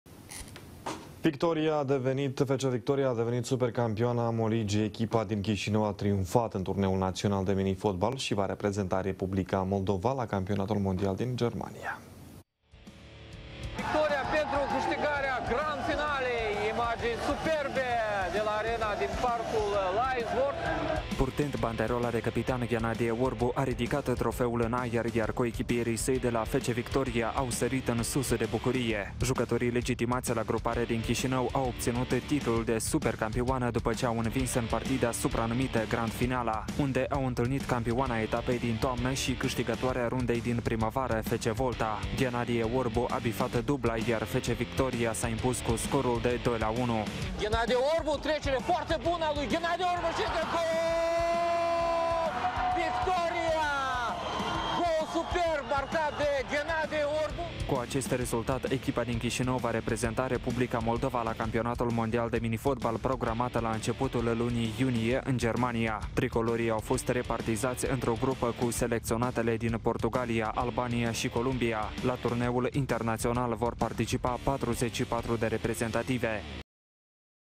FC Victoria a cucerit titlul suprem
FC Victoria a devenit super-campioana Amoligii. Echipa din Chișinău a triumfat în turneul național de minifotbal și va reprezenta Republica Moldova la Campionatul Mondial din Germania.